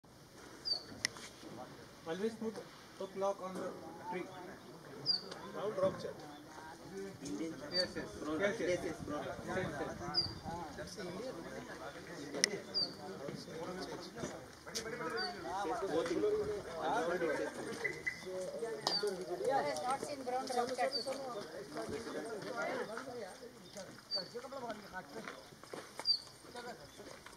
Brown Rock Chat Call
Earlier called the Indian Chat, the Brown Rock chat, Oenanthe fusca, is found in old buildings and rocky areas in Central India.
These birds have been known to feed late and forage on insects attracted to artificial lighting, and have nearly 8 different calls. We heard this call, the short whistle like chee chee

Seen at Bhimbhetka, near Bhopal

The Bhimbetka rock shelters are an archaeological site in central India that spans the Paleolithic and Mesolithic periods, as well as the historic period. It exhibits the earliest traces of human life in India and evidence of Stone Age starting at the site in Acheulian times.
These cave paintings show themes such as animals, early evidence of dance and hunting. The Bhimbetka site has the oldest-known rock art in India, as well as is one of the largest prehistoric complexes.

After the Ratapani Bird Survey.